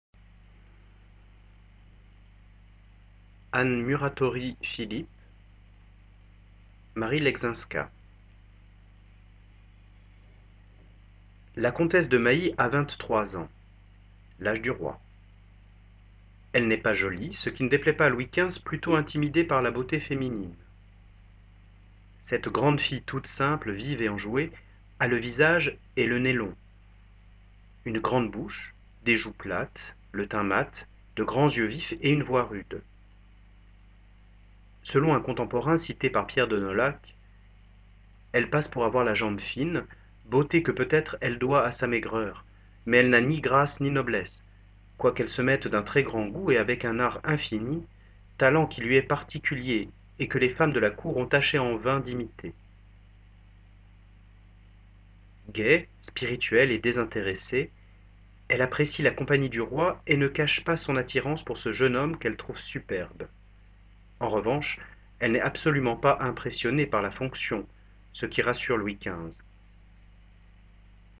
La comtesse de Mailly
Portrait de Louise Julie de Mailly-Nesle par Anne Muratori-Philipp dans "Marie Leszczynska".
Tableau de Alexis Grimou.
Lecteur : David-Georges Picard / Gaudium libris